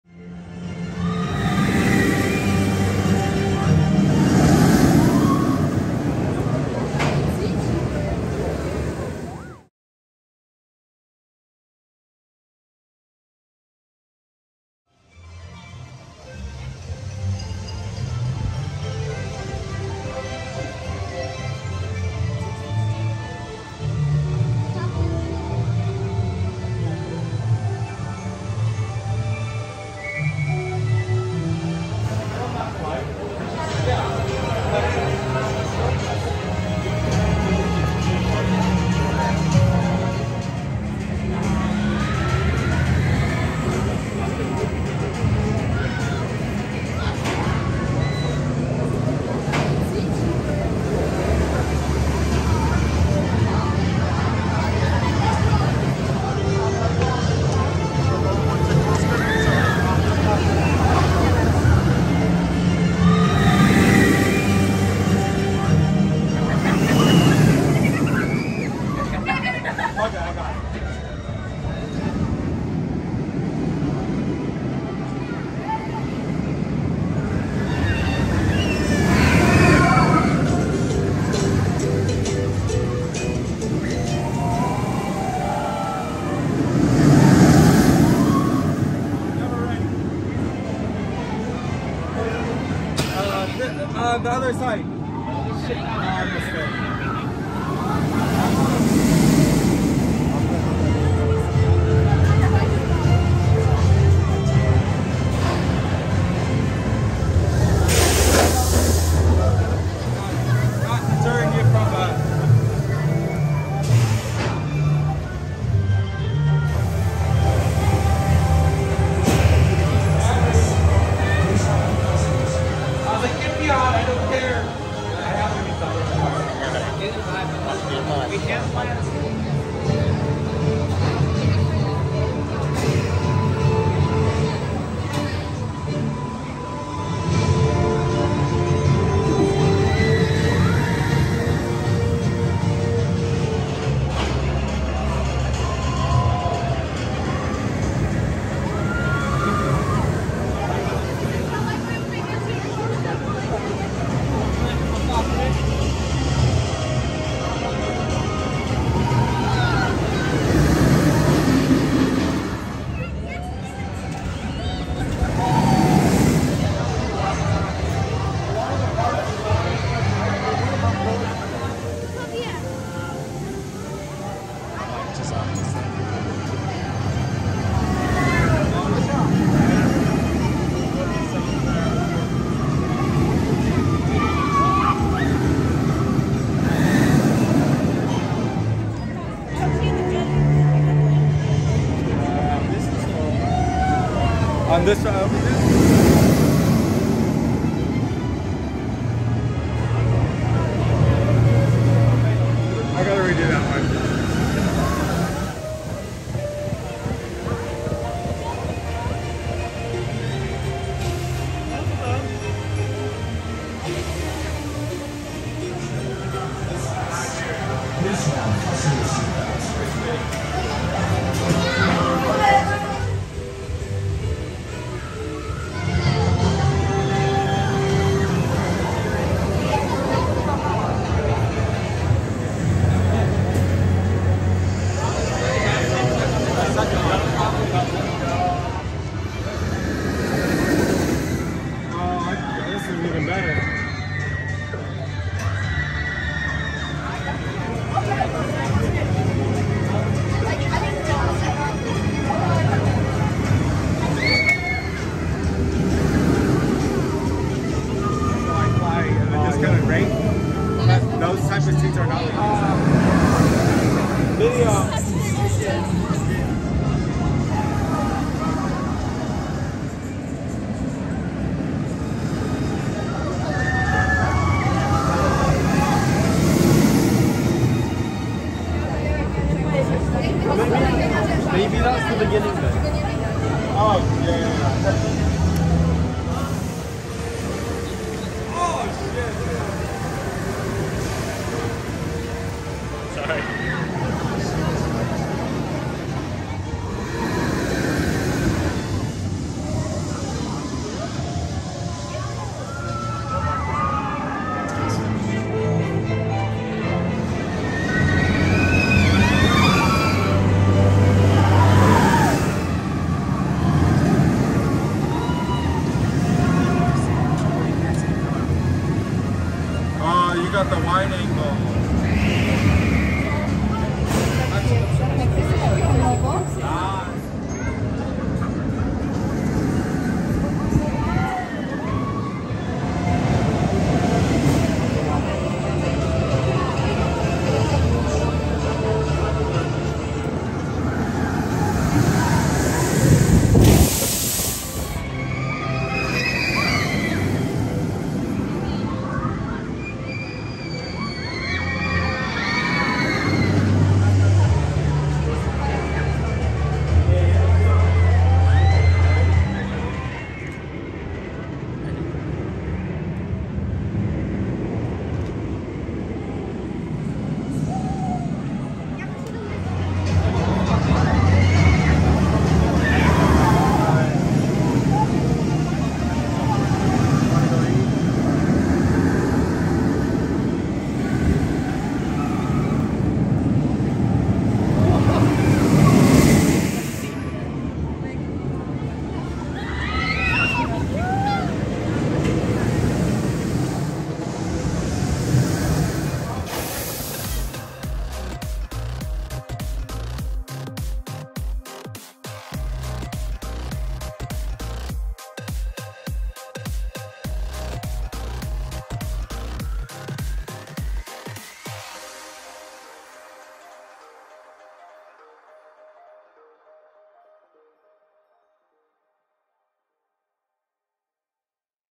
Abyssus Off-Ride Energylandia 2022 \\ 4K60FPS - No Copyright
Check out this NO COPYRIGHT footage of Abyssus located at Energylandia!! This Vekoma Shockwave opened in 2021 and is one of Energylandia's great coasters!!